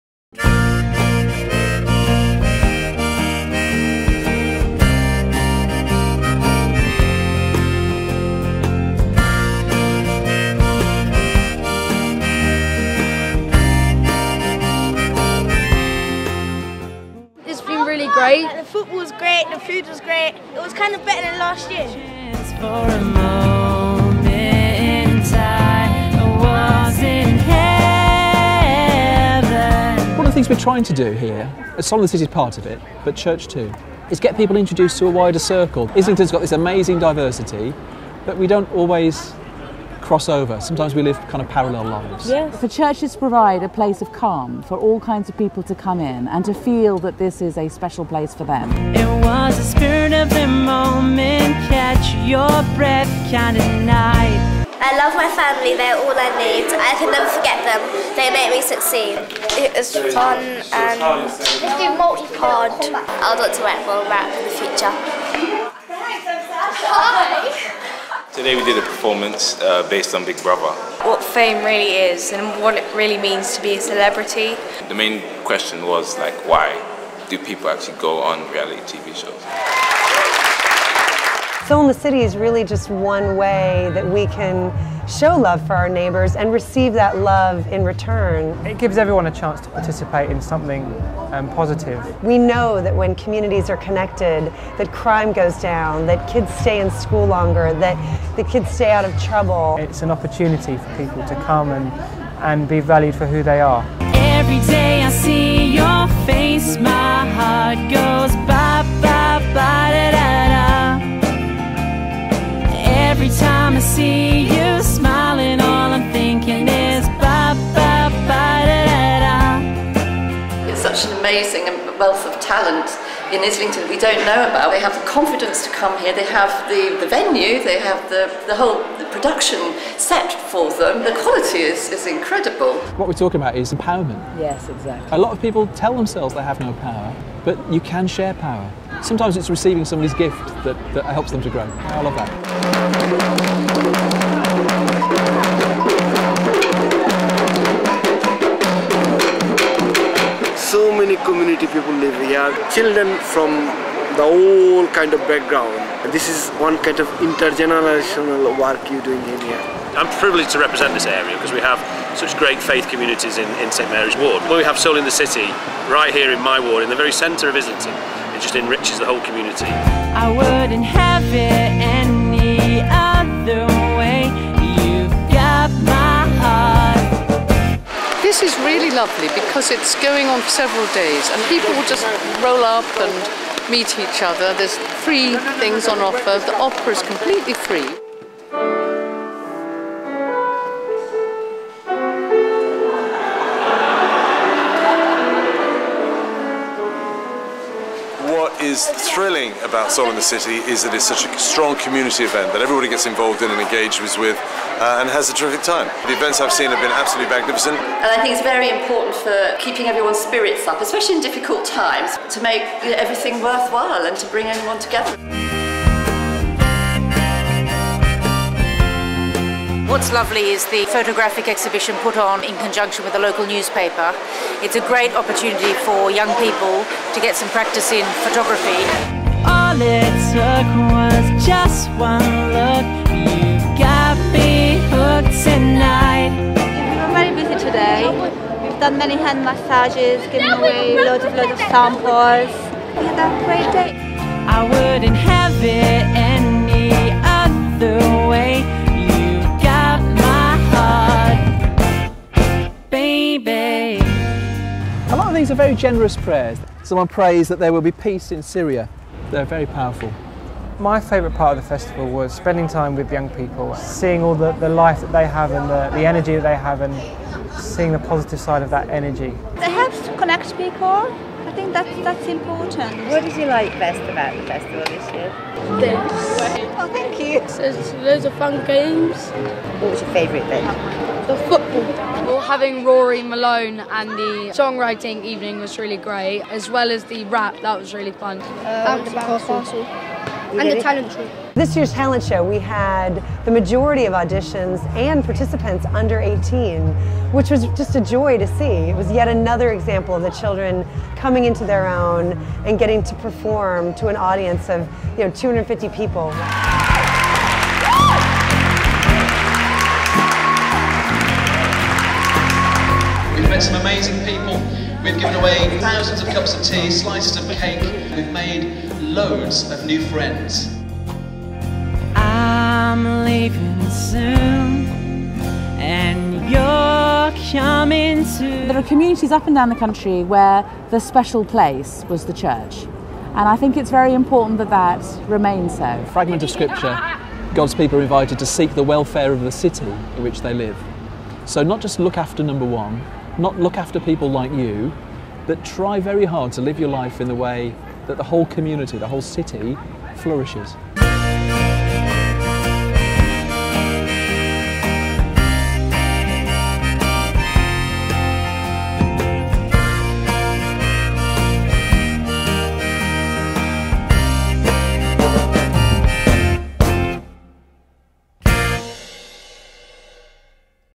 Soul in the City 2012
For six days in June 2012, St Mary Islington held a neighbourhood festival, bringing together the communities of one of the most exciting areas of London. Despite Upper Street's reputation for shops and restaurants, our parish is one of the most deprived in the country. Soul in the City 2012 showed that when people meet, friendships are forged and hope for a better future together can be born.